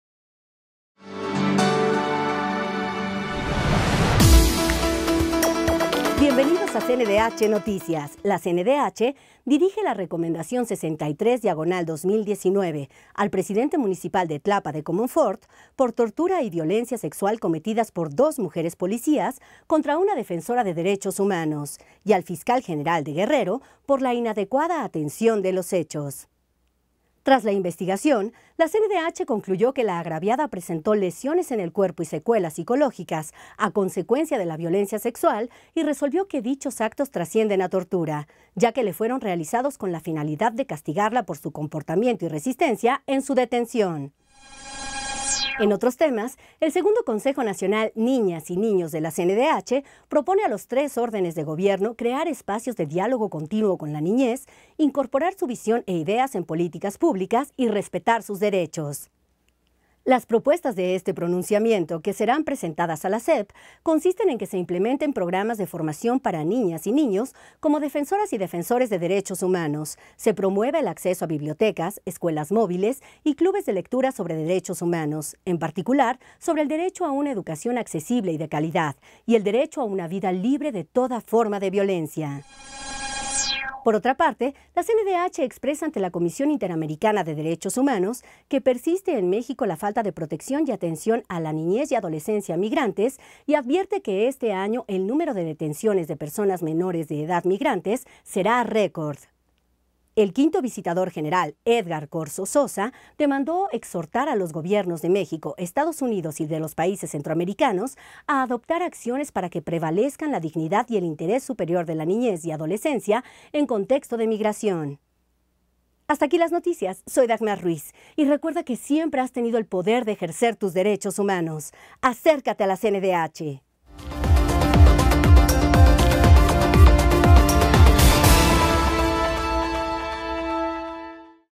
CNDH NOTICIAS (Programa 270)
La CNDH dirige la Recomendación 63/2019 al Presidente Municipal de Tlapa de Comonfort, por tortura y violencia sexual cometidas por dos mujeres policías contra una defensora de derechos humanos, y al Fiscal General de Guerrero por la inadecuada atención de los hechos / El Segundo Consejo Nacional Niñas y Niños de la CNDH propone a los tres órdenes de gobiernos crear espacios de diálogo continuo con la niñez, incorporar su visión e ideas en políticas públicas y respetar sus derechos / La CNDH expresa ante la Comisión Interamericana de Derechos Humanos que persiste en México la falta de protección y atención a la niñez y adolescencia migrantes y advierte que este año el número de detenciones de personas menores de edad migrantes será récord.